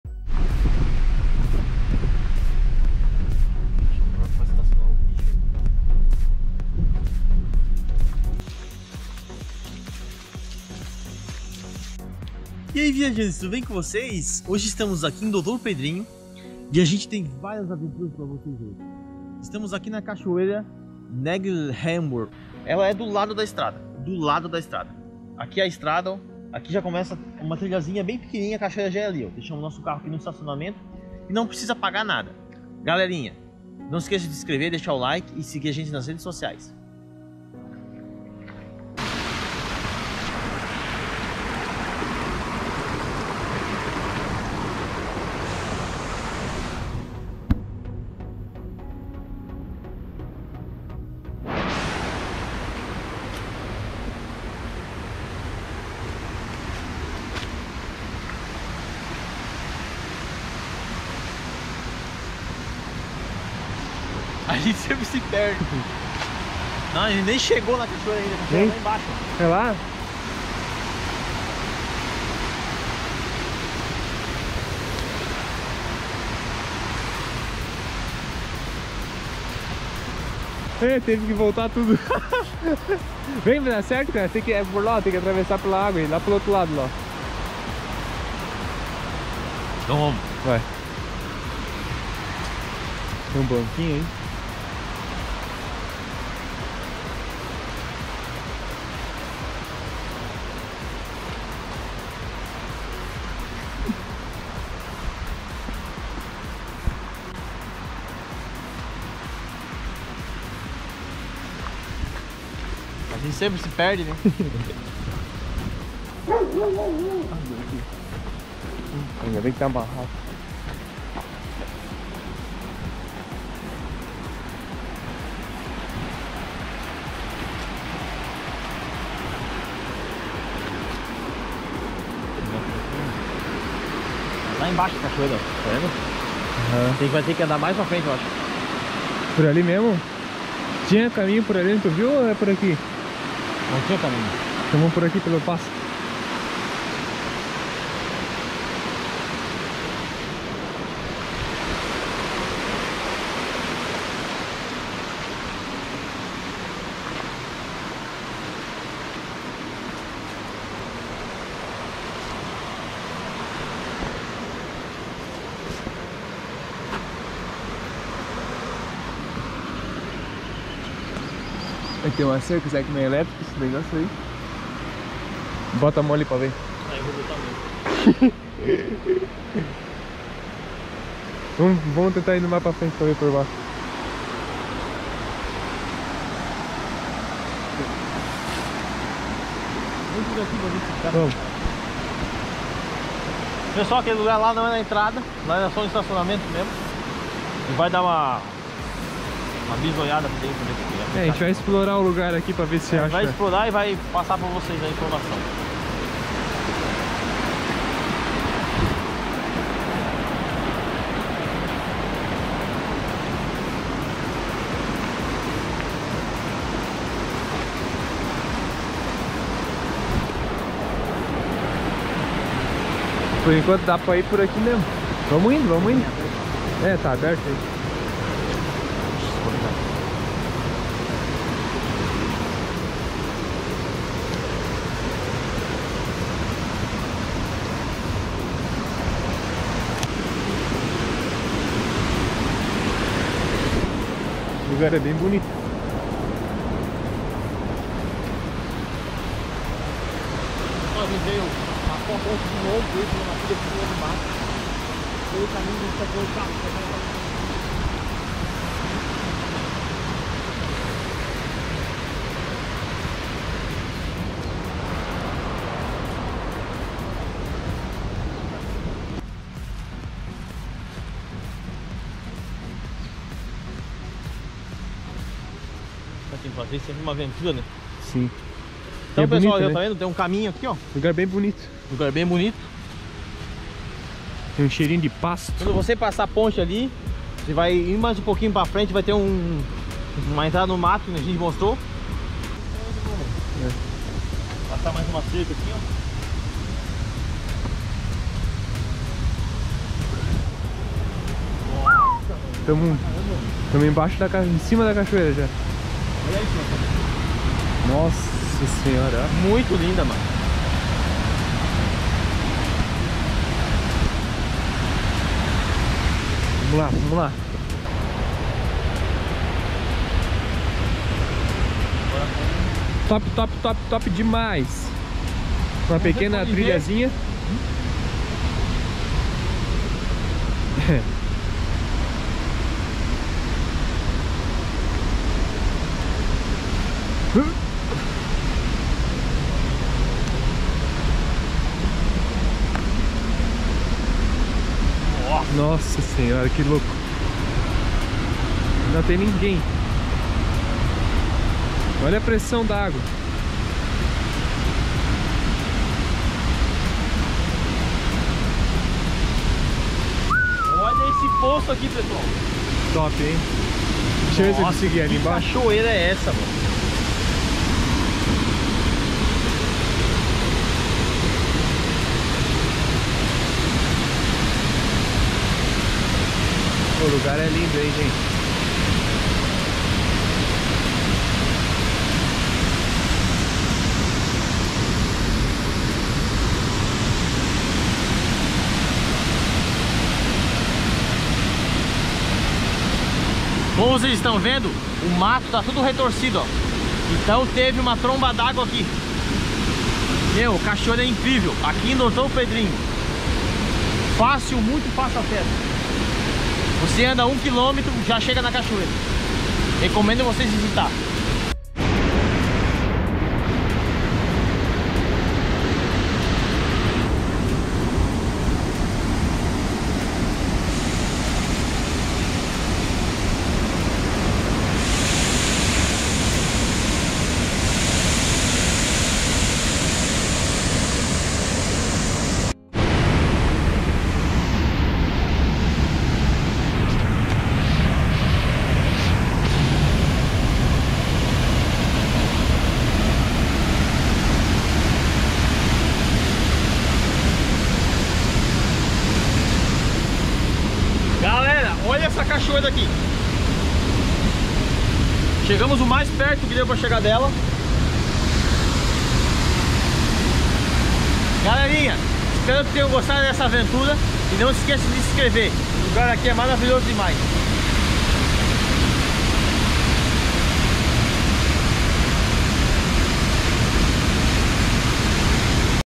CACHOEIRA INCRIVEL EM DOUTOR PEDRINHO /SC - CACHOEIRA DO NEGHERBON
olá viajantes olha só essa cachoeira que encontramos em doutor Pedrinho simplesmente fantástica
se for ir não jogue lixo preserve natureza